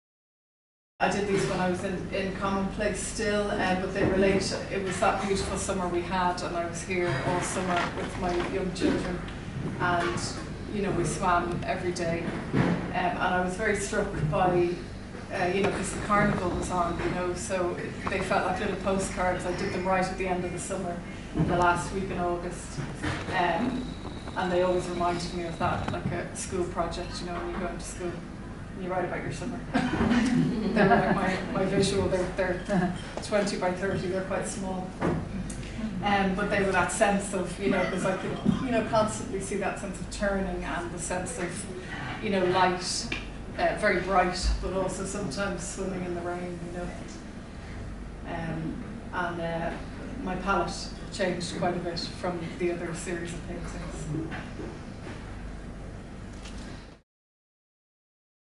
Joanne Boyle @ bray arts show
short video of Joanne Boyle presenting her work
@ bray arts show,

January 2016